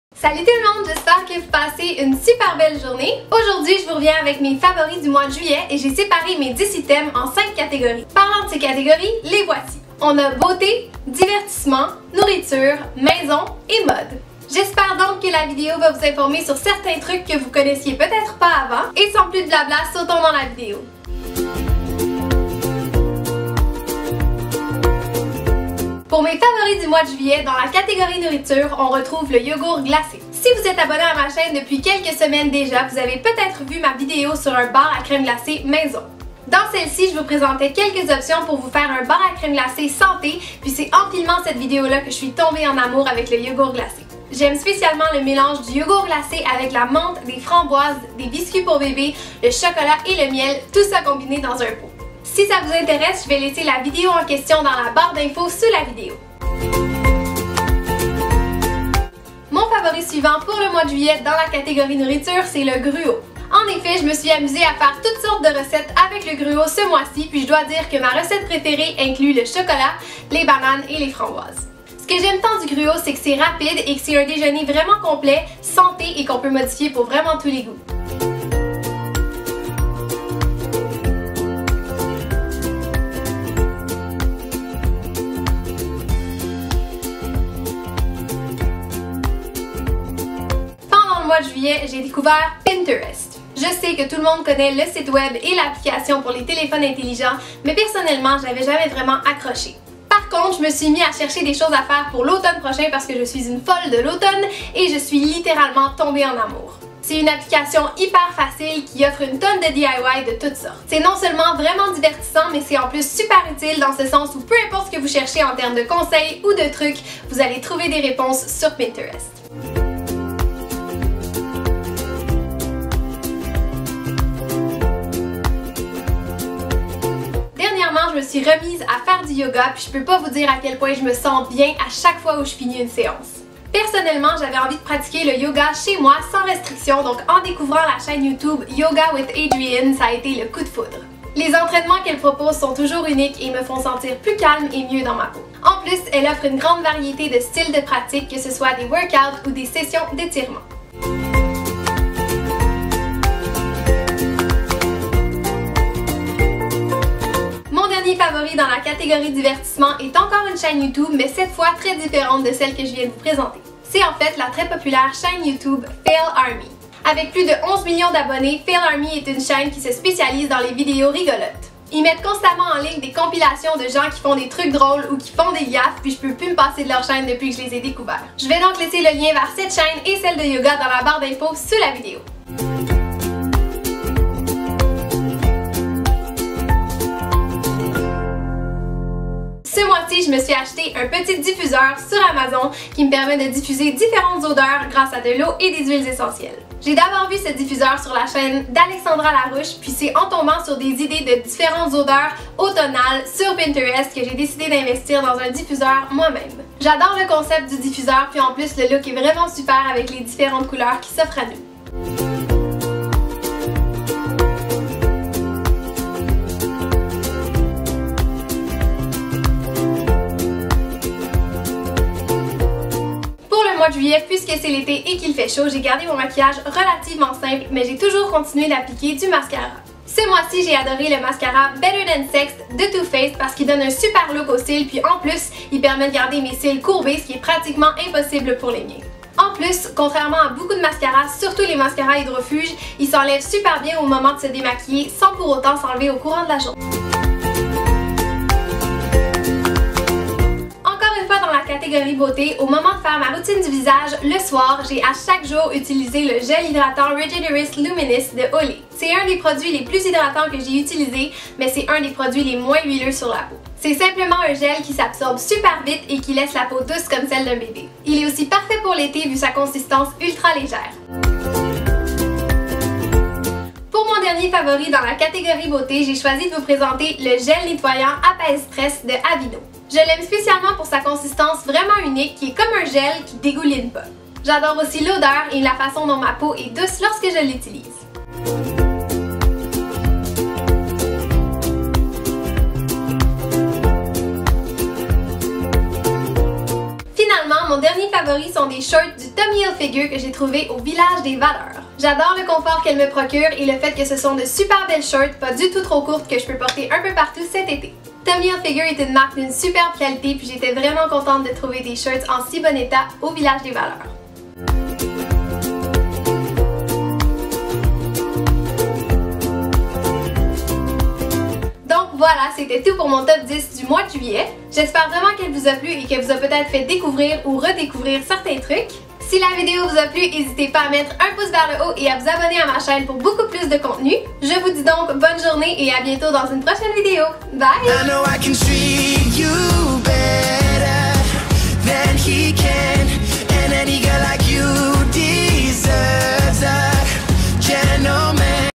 TOP 10 de Juillet | Favoris 2016
OUVRE LA BOITE D'INFO et regarde la vidéo en HD (hihi)

Dans cette vidéo je vous présente mes favoris du mois de juillet!!

J'espère que la vidéo vous plaira et qu'elle vous informeras sur quelques éléments de mon top 10 de juillet!!!

-Maude xx

OU ME TROUVER - Viens dire bonjour!!